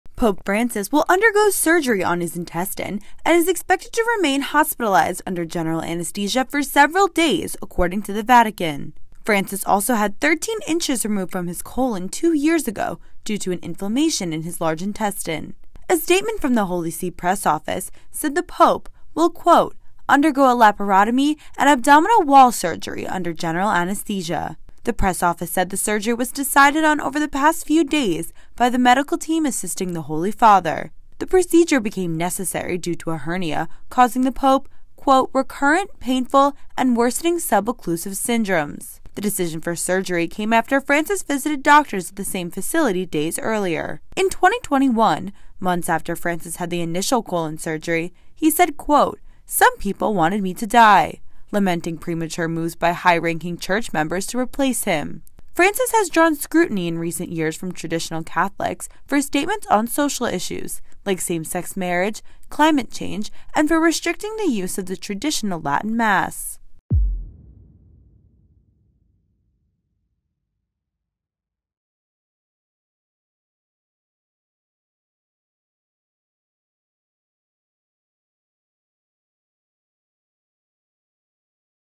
Pope Francis taken to hospital to undergo intestinal surgery
Pope Francis will undergo surgery on his intestine Wednesday afternoon and is expected to remain hospitalized under general anesthesia for several days, the Vatican said.

Francis, 86, had 13 inches removed from his colon two years ago due to an inflammation in his large intestine.